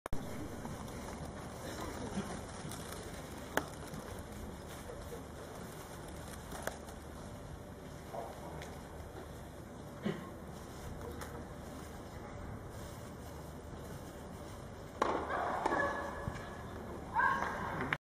Madrid Open 2010 - Williams-Rezai
Madrid Open, mayo de 2010. Venus Williams contra Arevane Rezai.